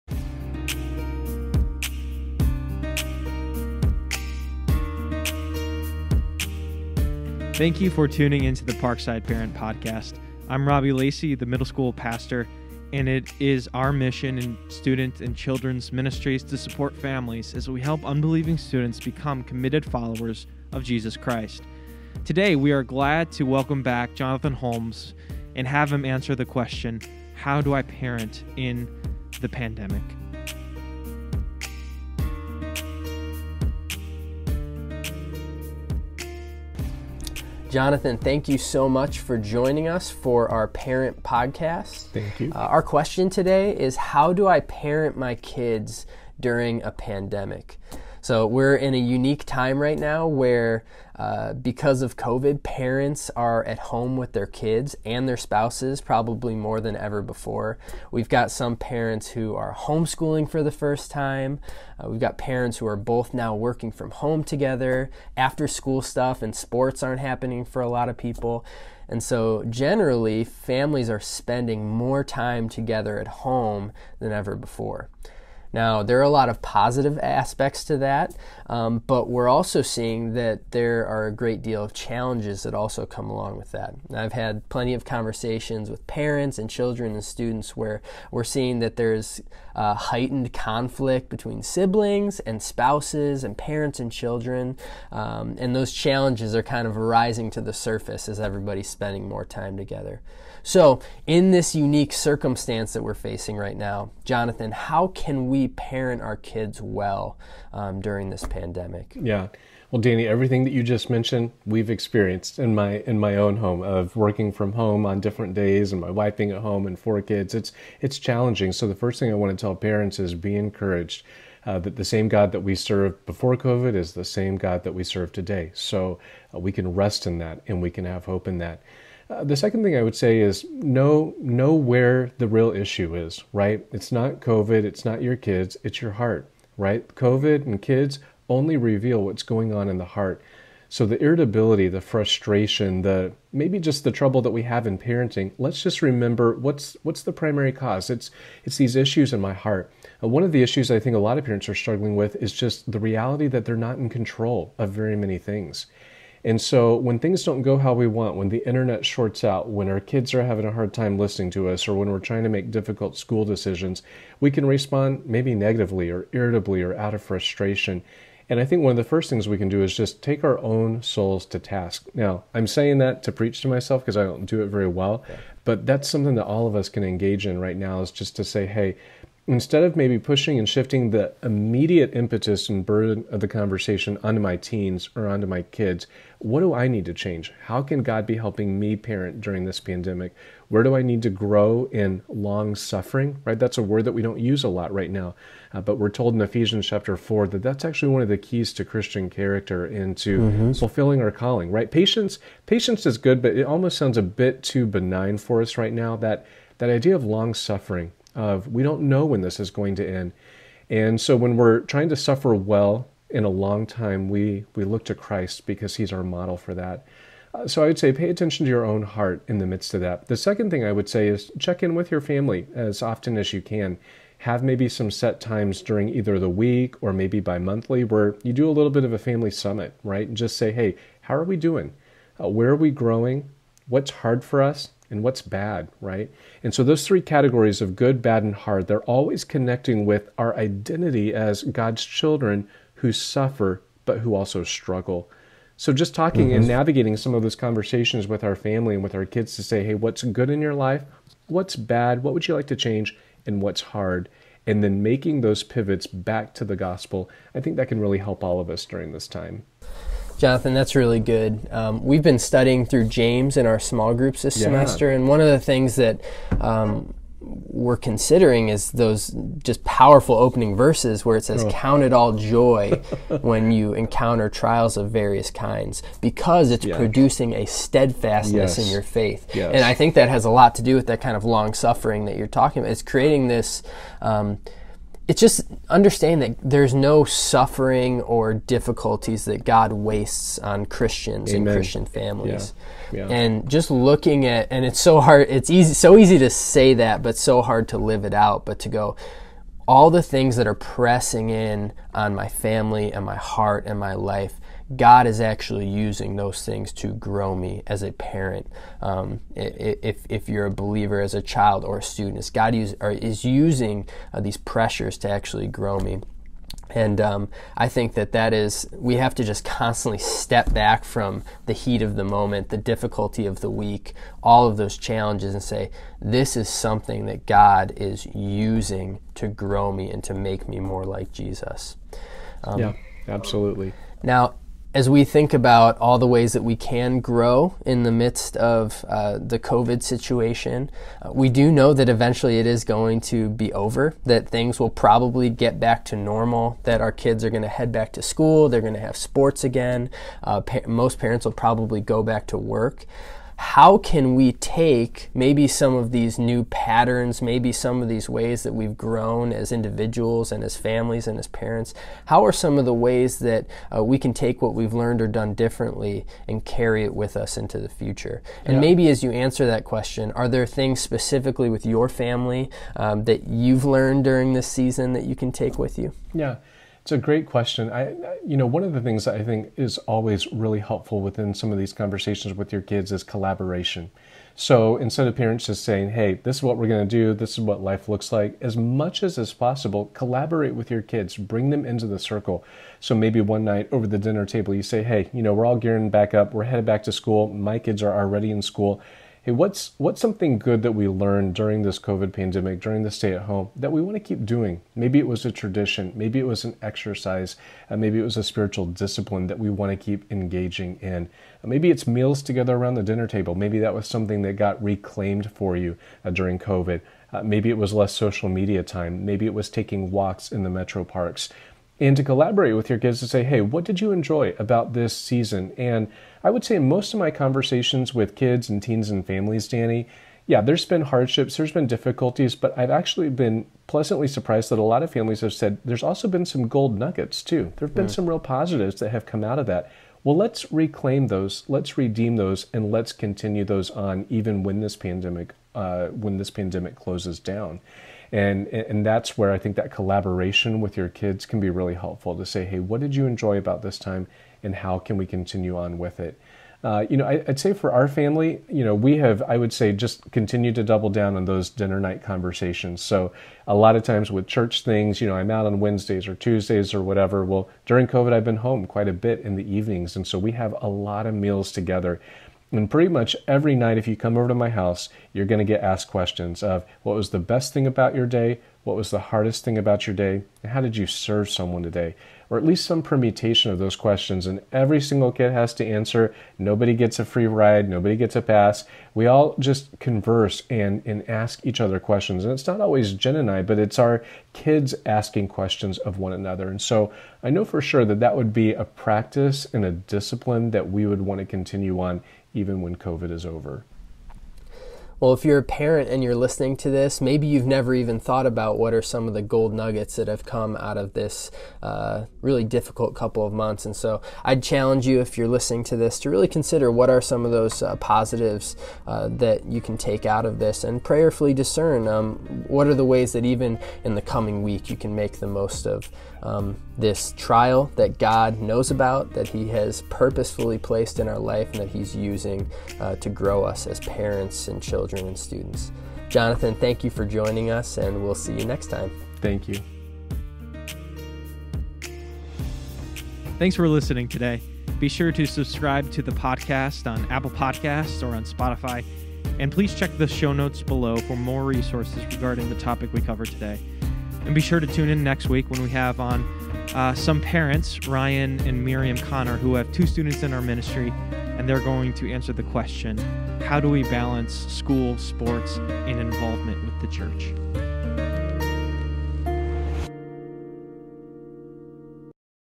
The Parkside Parent Podcast - Ep. 9 - Jonathan Holmes
Jonathan Holmes answers the question, "How do I parent in a pandemic?"